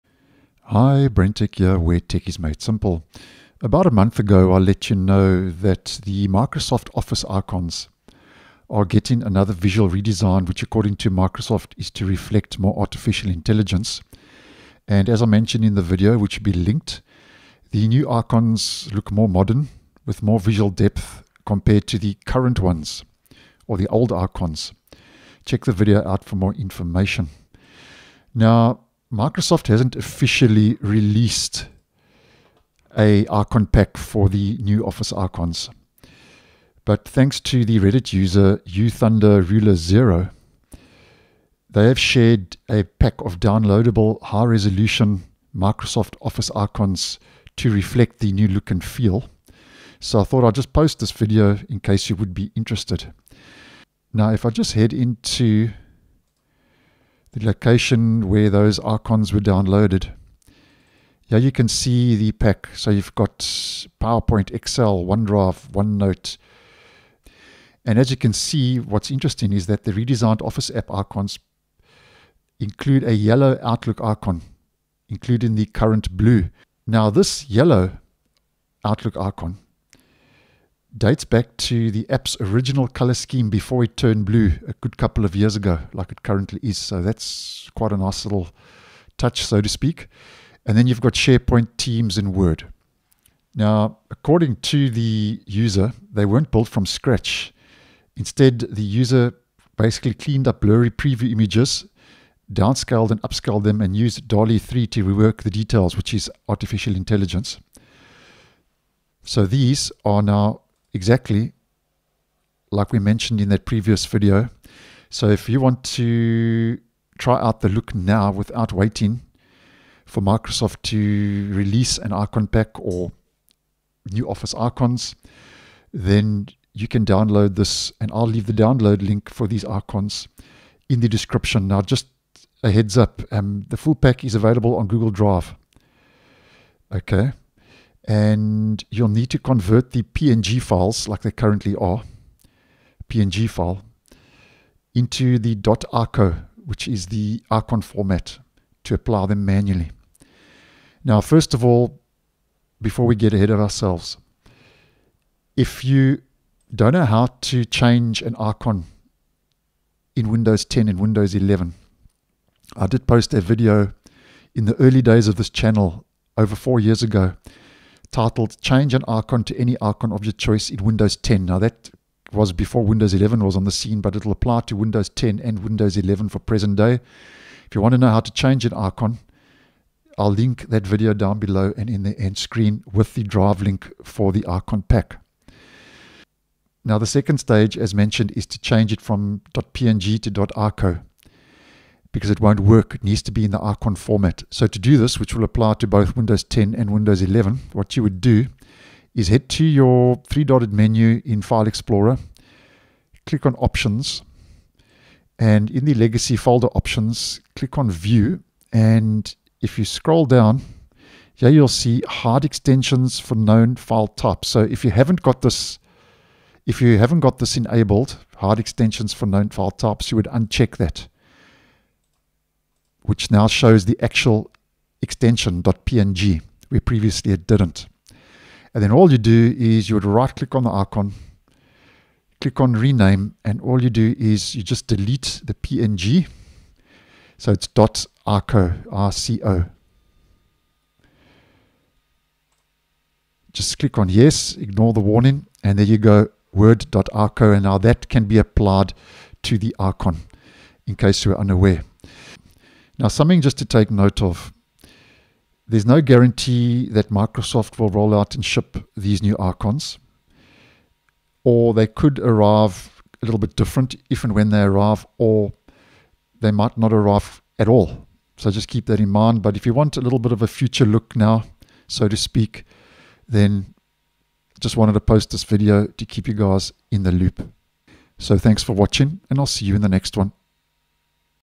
Download Microsoft's New Redesigned Office Icons Pack + Original Yellow Outlook
The full pack is available on Google Drive, but you’ll need to convert the PNG files into .ICO format to apply them manually